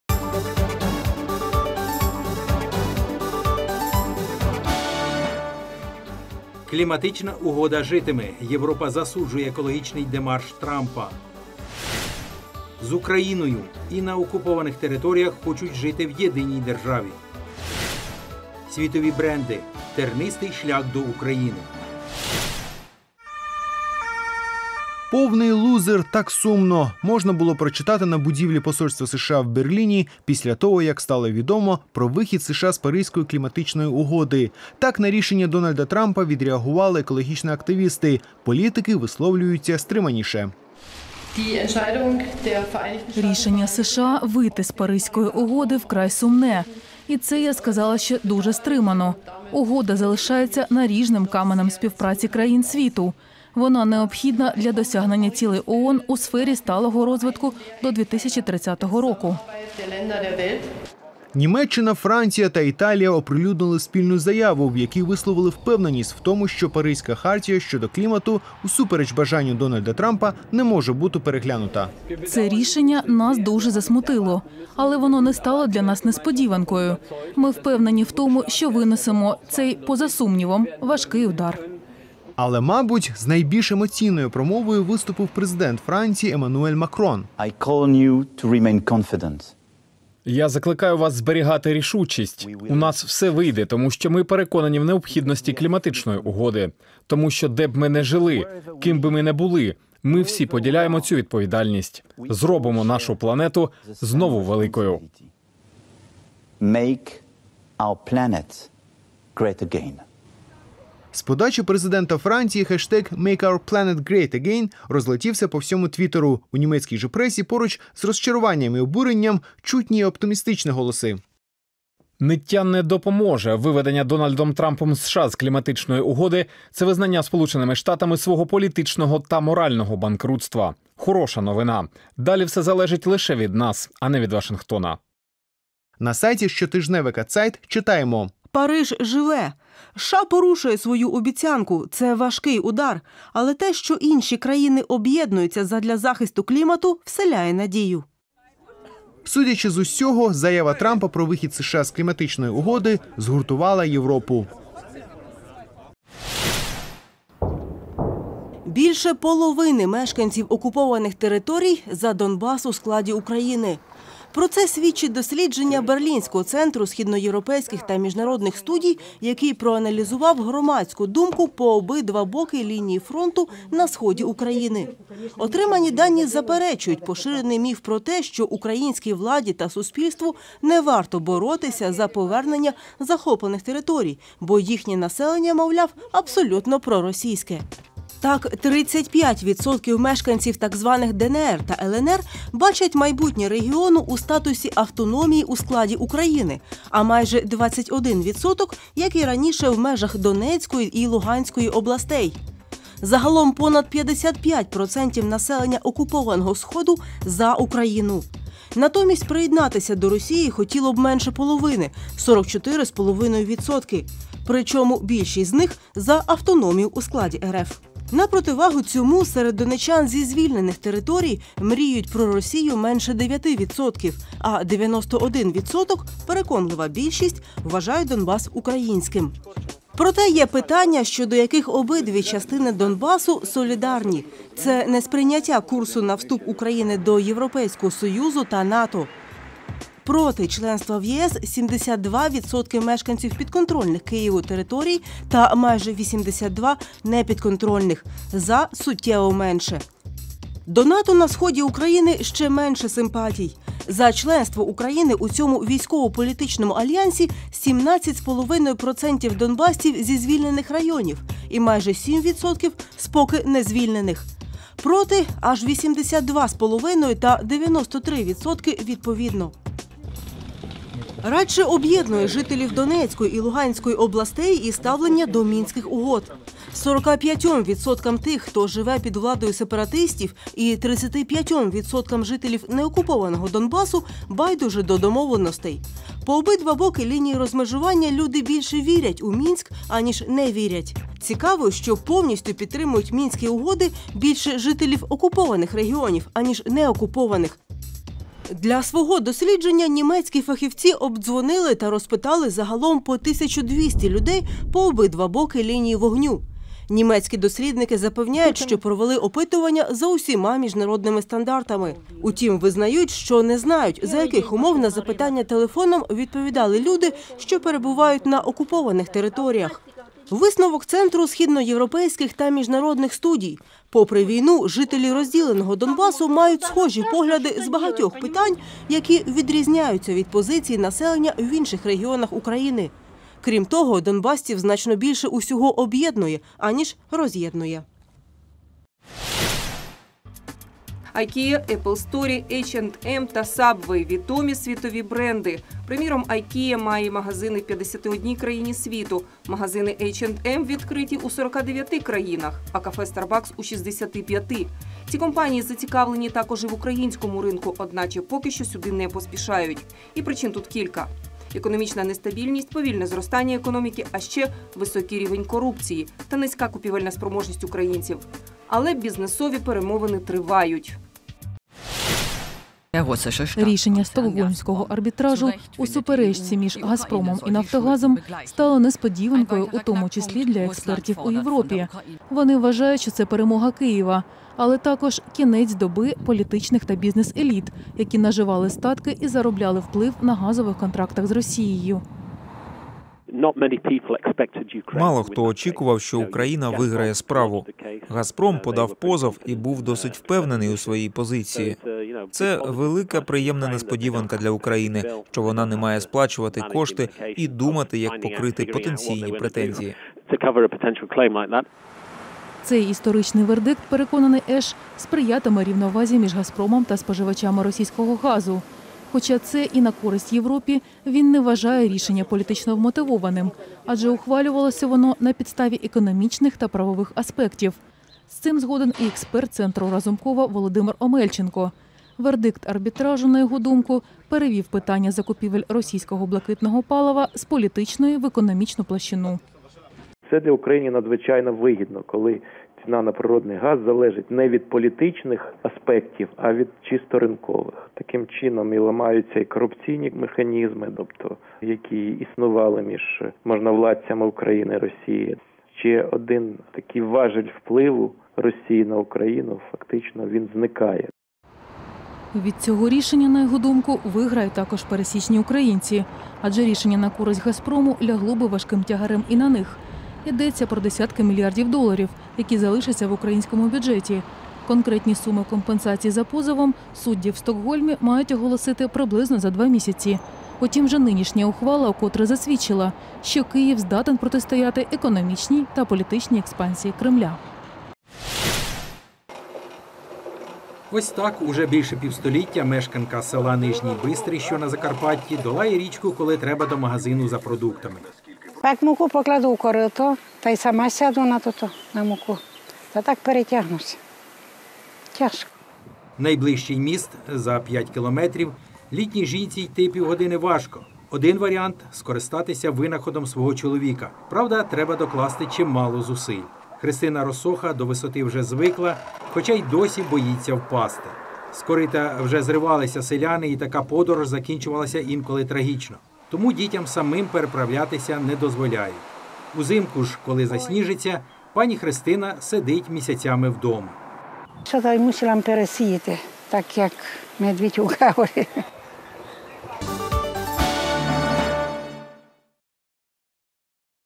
Донбас хоче бути в Україні. Вихід США з Паризької угоди. Візерунок дня (2.06.2017) | DW Ukrainian
Теми дня:
Як реагує світ на екологічний демарш Трампа
Берлінське дослідження: мешканці окупованих територій бачать Донбас у складі України
Apple, Starbucks, Ikea: чому світові бренди не йдуть в Україну
Мешканка закарпатського села понад півстоліття переходить через річку верхи на кориті